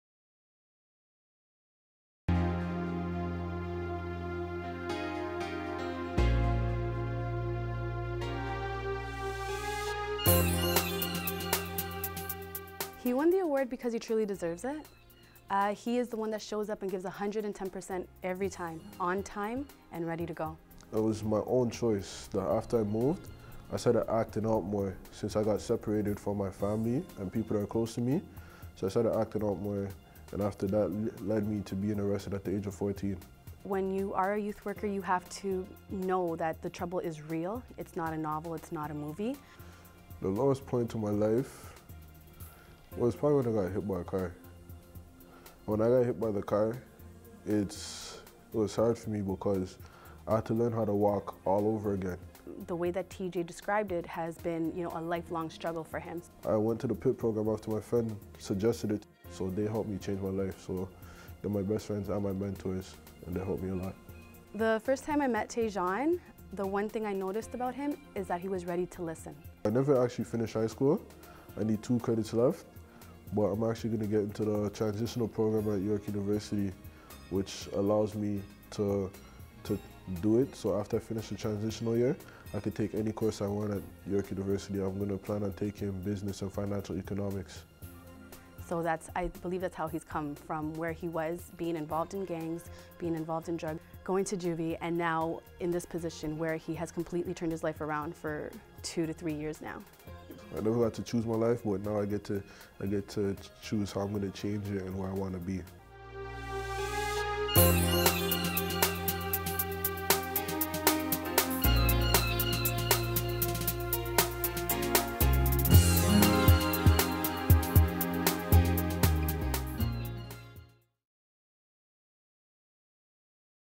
J12 JVS 2011 Youth Award Taejon Wallace
Interview with JVS 2011 Youth Award recipient Taejon Wallace - his story and achievements.